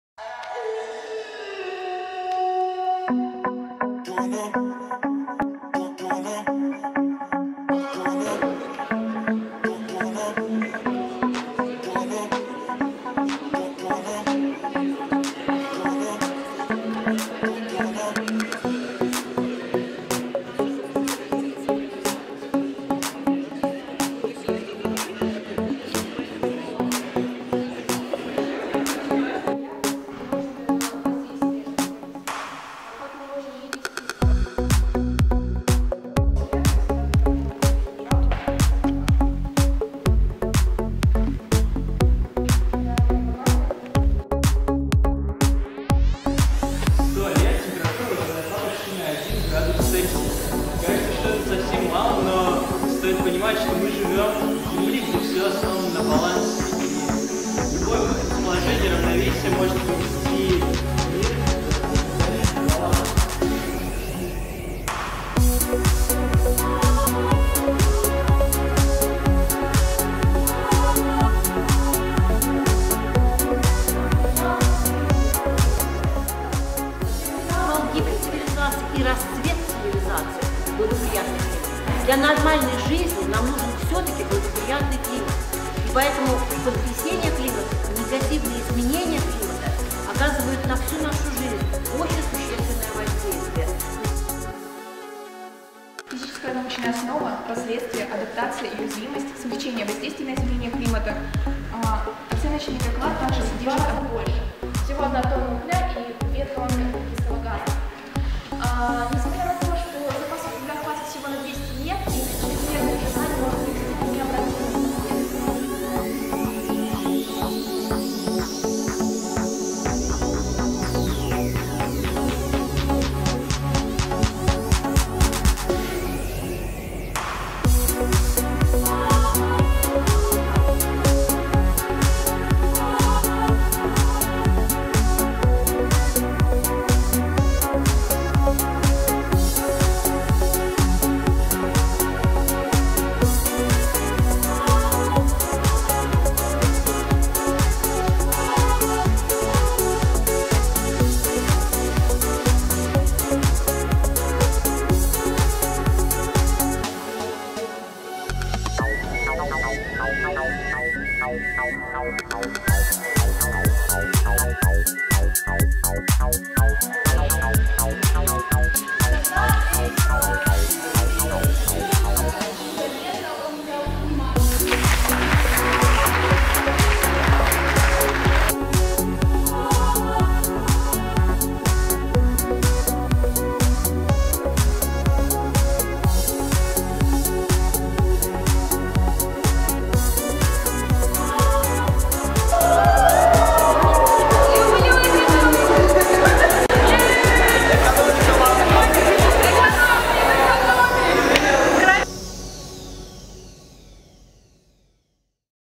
Глобальные риски XXI века: здоровье планеты, здоровье человека
3-4 декабря 2016 в ПСПБГМУ имени академика И.П. Павлова состоялась конференция "ГЛОБАЛЬНЫЕ РИСКИ XXI века:  здоровье планеты, здоровье человека"